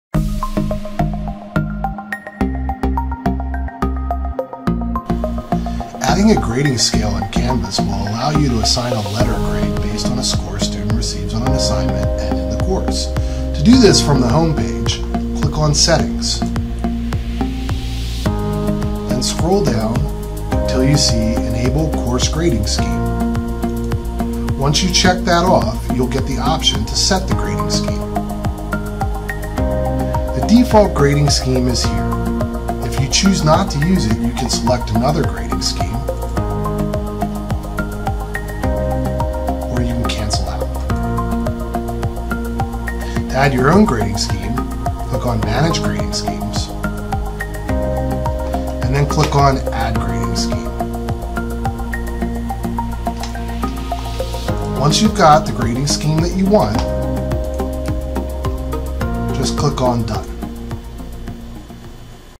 Grade Scheme in Canvas
How to select a grading scheme in canvas for letter grading